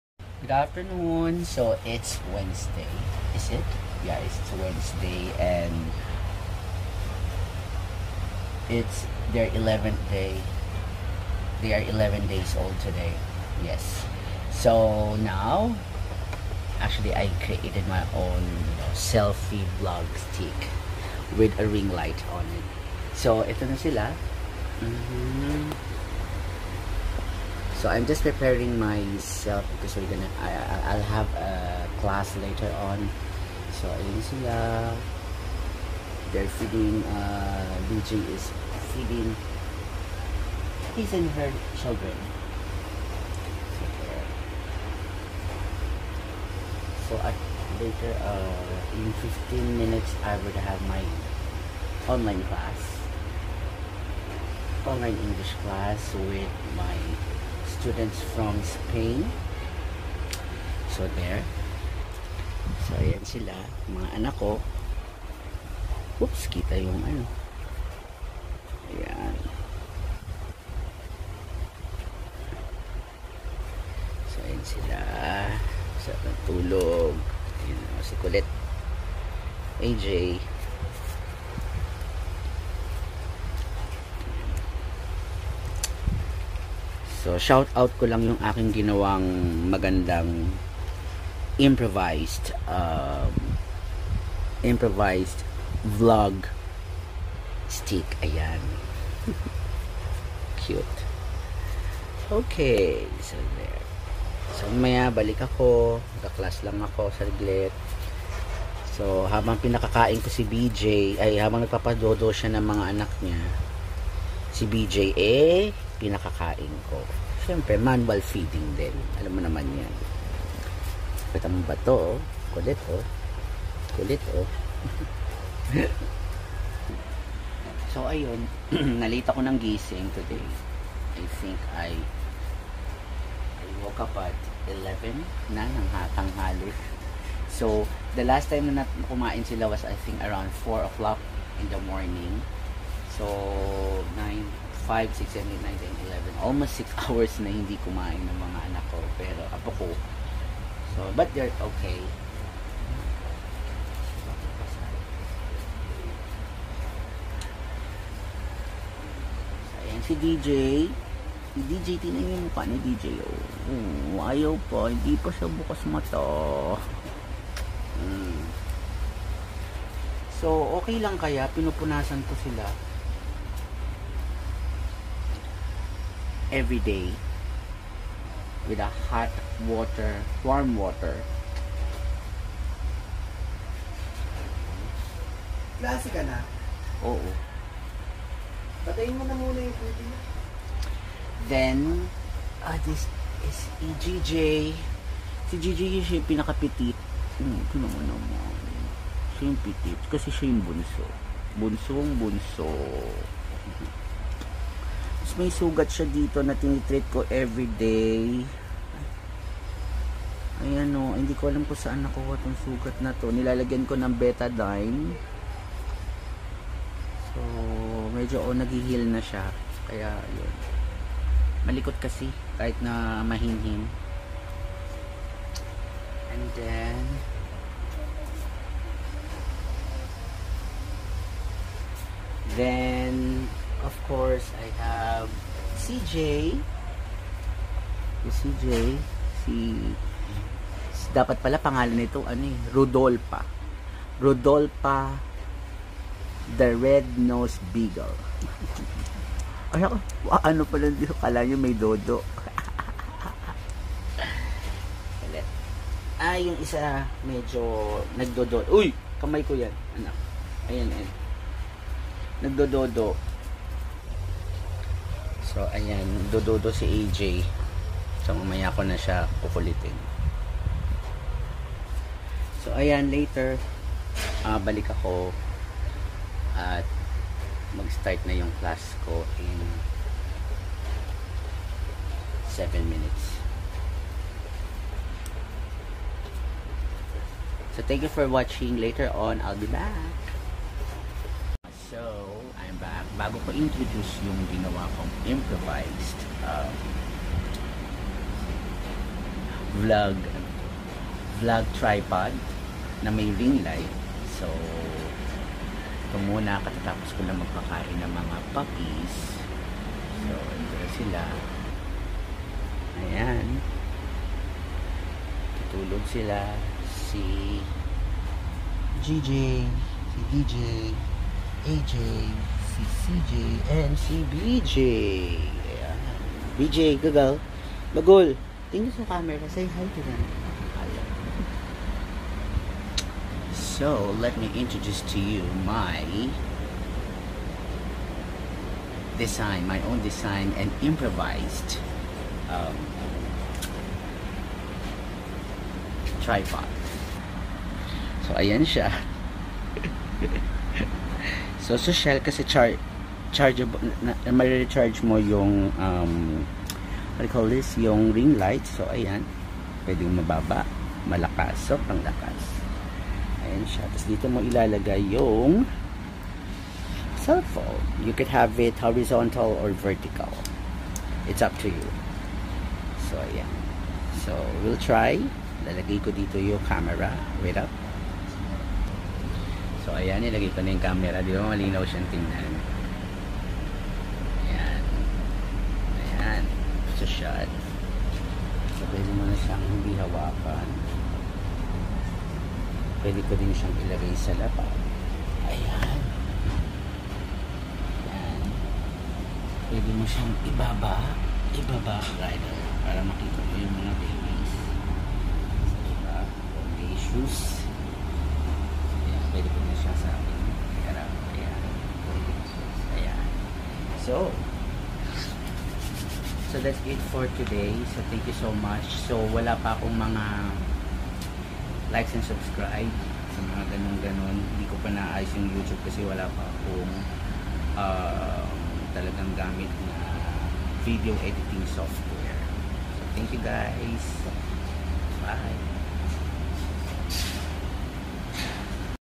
9.9.2020 Beagle serye... 11 days old of "Fantastic 4Js"
In the video, I included my customised and improvised mobile Tripod with ringlight.. hahaha